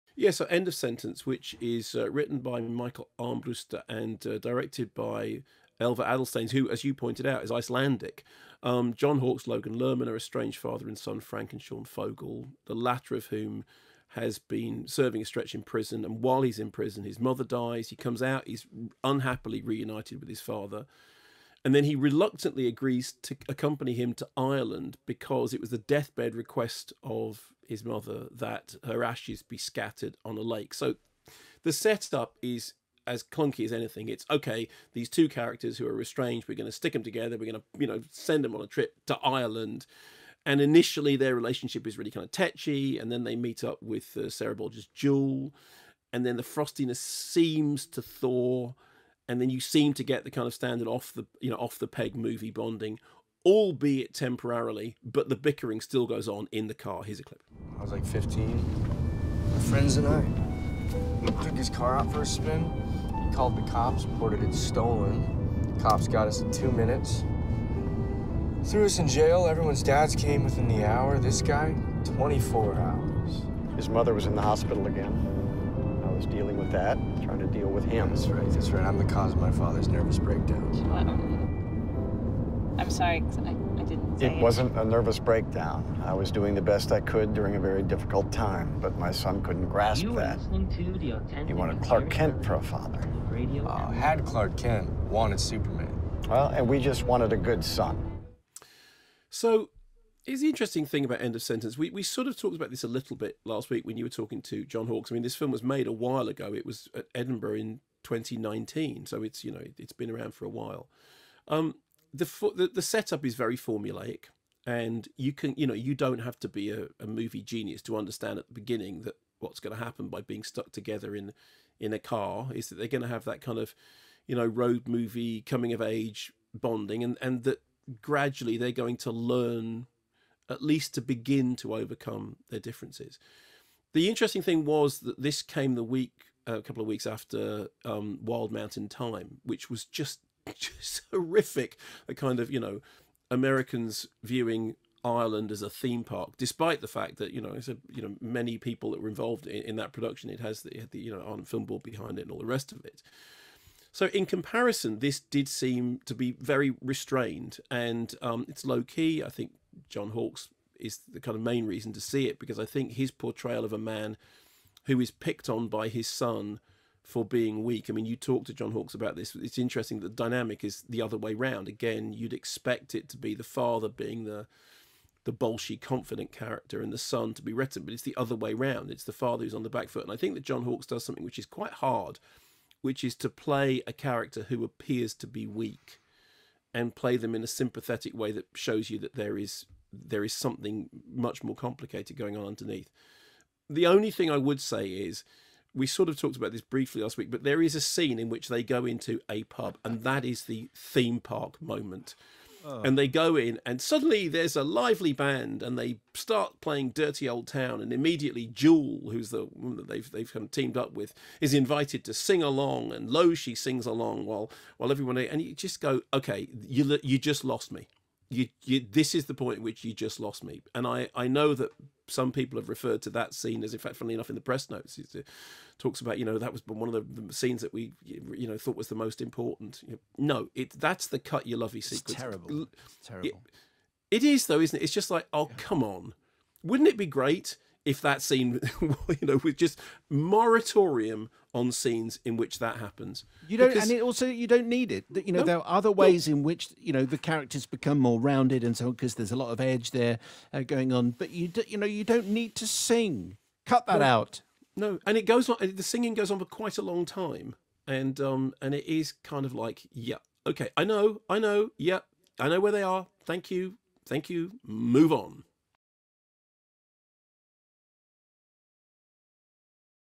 End of Sentence reviewed by Mark Kermode
Mark Kermode reviews End of Sentence. A father convinces his estranged son to travel with him to Ireland, to fulfil his wife's wish to have her ashes scattered there.